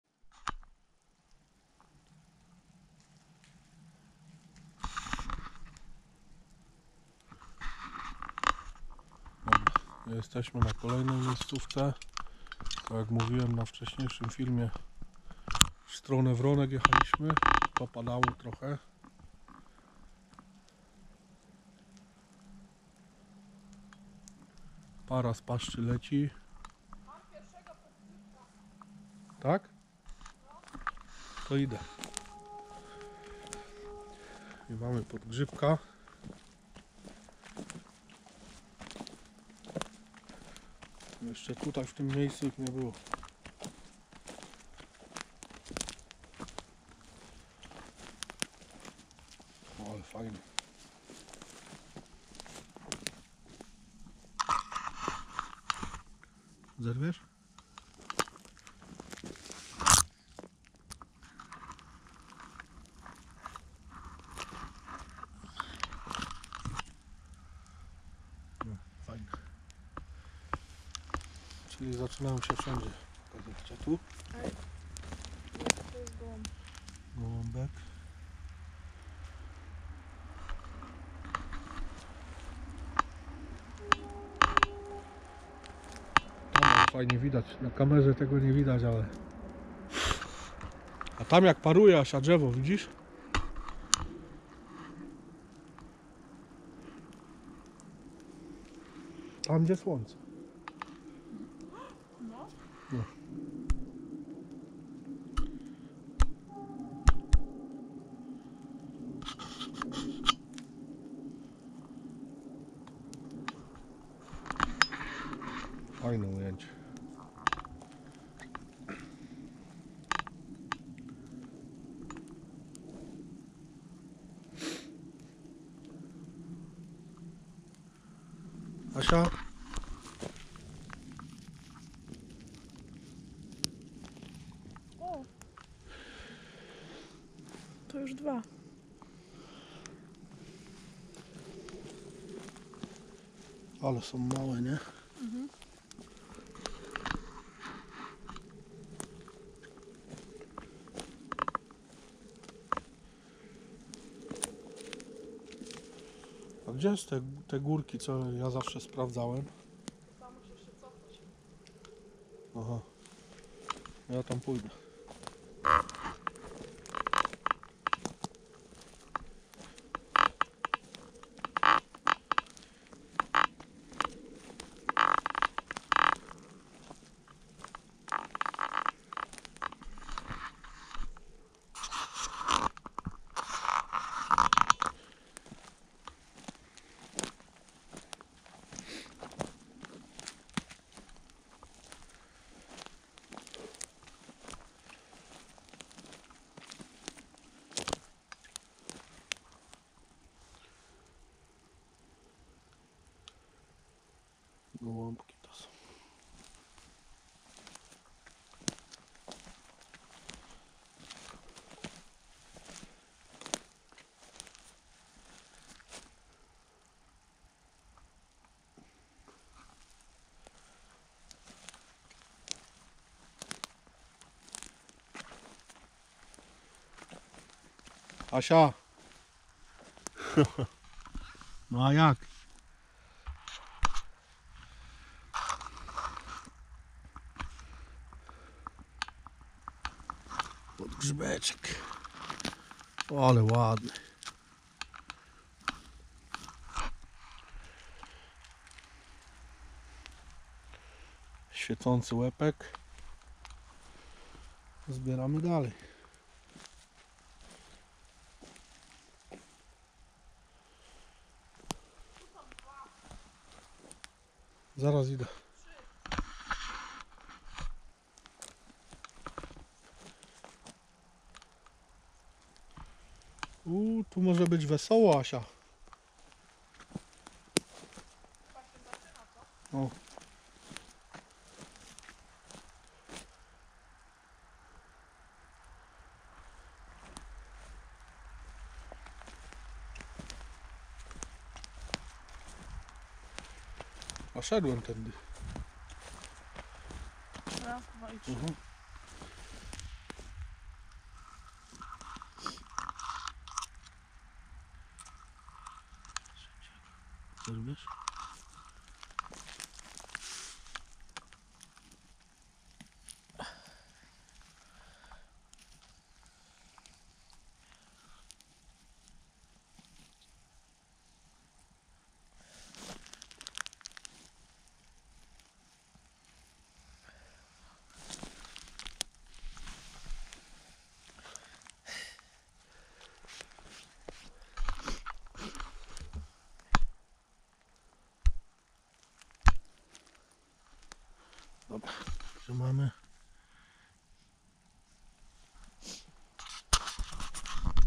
Podgrzybki z Chojeńskiej Puszczy Noteckiej.
Tym razem okolice Chojna i kolejne podgrzybki.